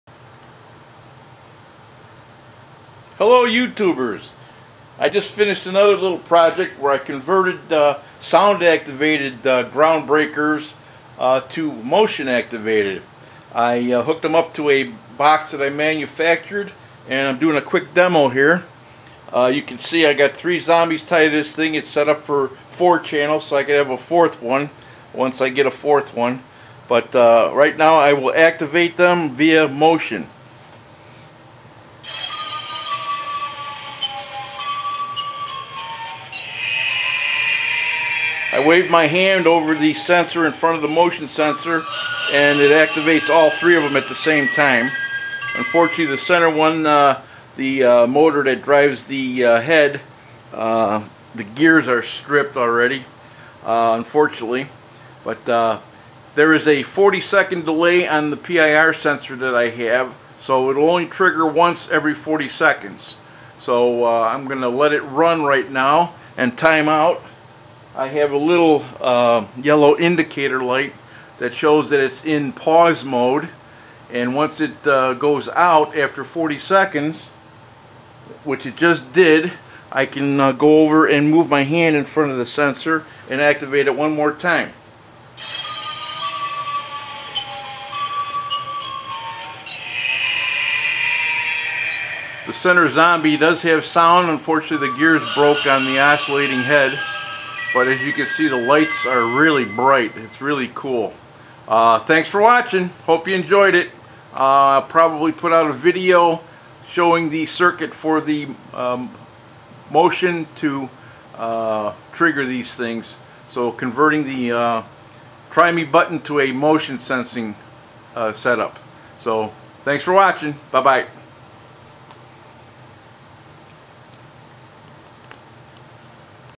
Halloween TRY ME button to HC-SR501 PIR Motion Trigger conversion demo
This is a demonstration of 3 Zombie ground breaker animatronics with a circuit I build to convert the "TRY ME" button circuit to a  HC-SR501 PIR  Motion sensing trigger.   This trigger can be used for any animatronic that has a Try Me button on it.   These Zombies are designed for sound activation but in a yard display motion activation is more practical so I came up with this conversion.  The trigger waits for motion then triggers all 3 Zombies simultaneously.   The PIR has a pause delay between triggers so I used that to have a 40 second pause between shows so folks don't continuously re-trigger the show.    This pause is adjustable from 2 seconds to 3 minutes or so.  If you are interested leave me a comment and I will make a video showing the circuit with schematic so you can build your own PRI Motion Trigger for your Halloween or Christmas animatronic displays.   Happy Haunting!!!